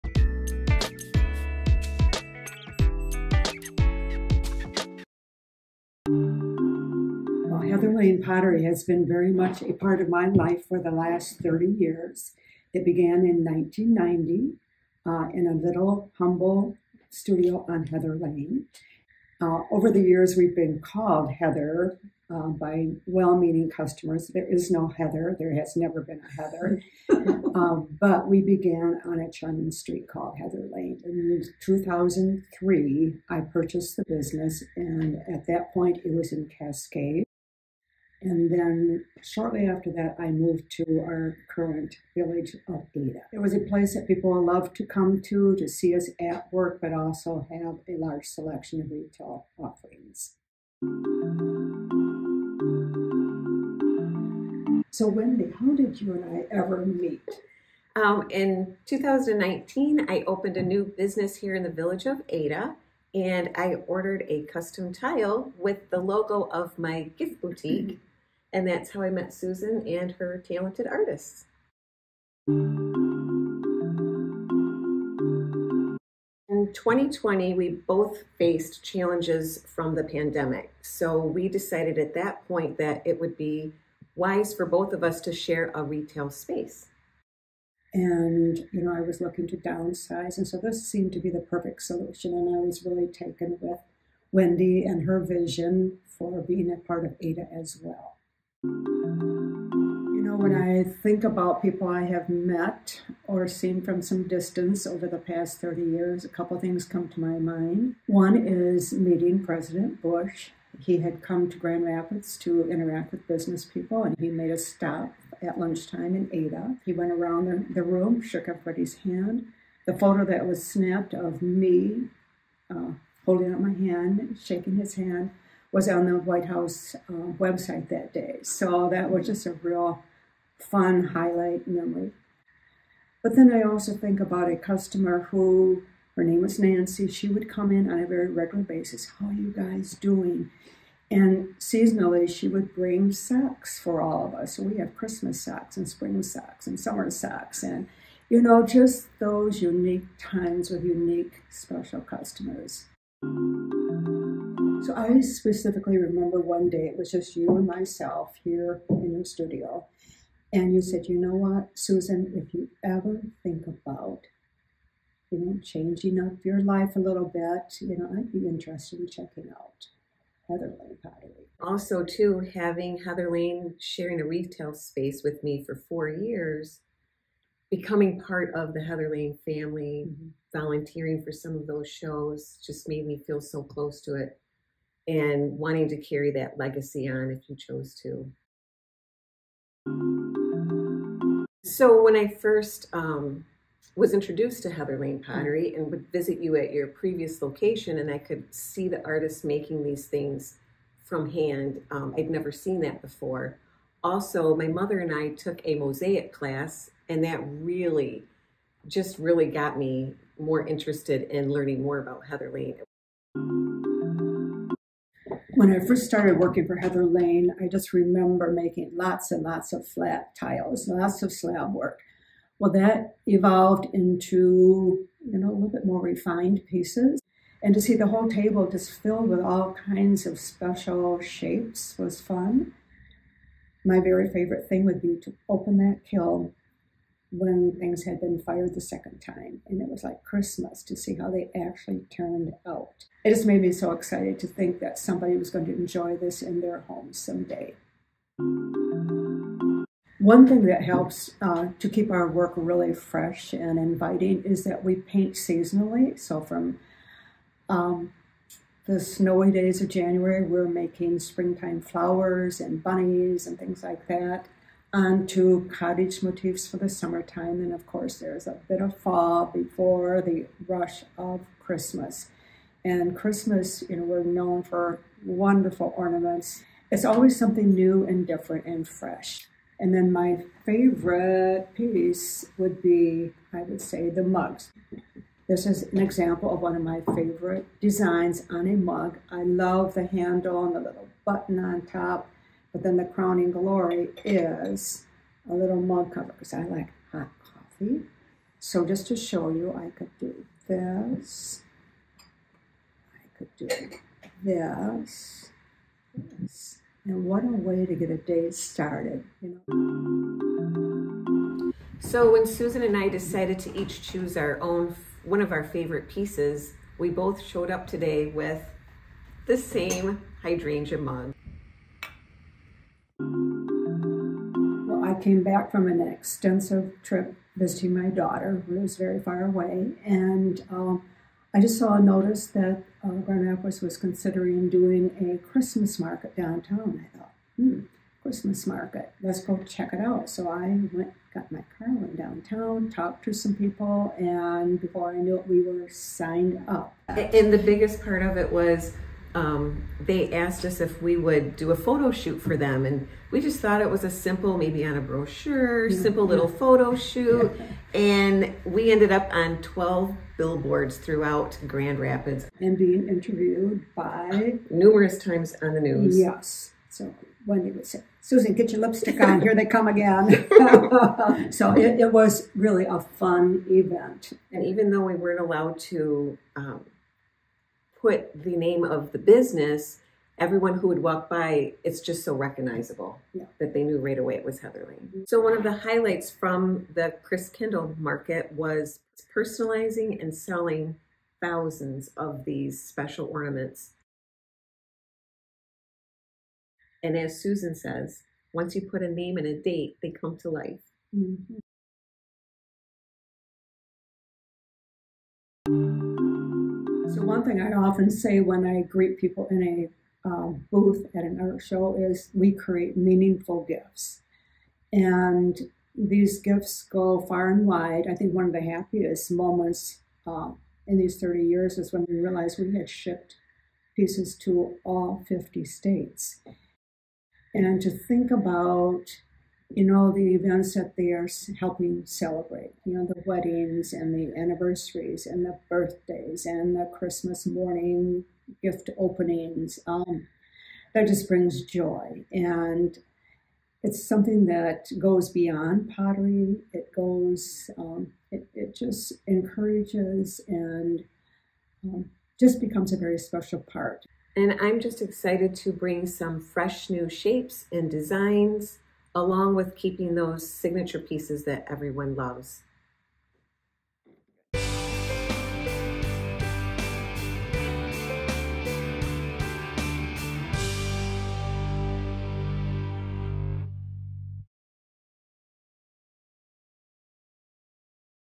A little interview with Susan & Wendy
As Susan moves on to the next phase of her life after years of dedication to cultivating the business of Heather Lane Pottery and as Wendy steps in to her new role as owner, we wanted to create a little interview video so our customers could get a little taste of the history, legacy and why we love this business so much!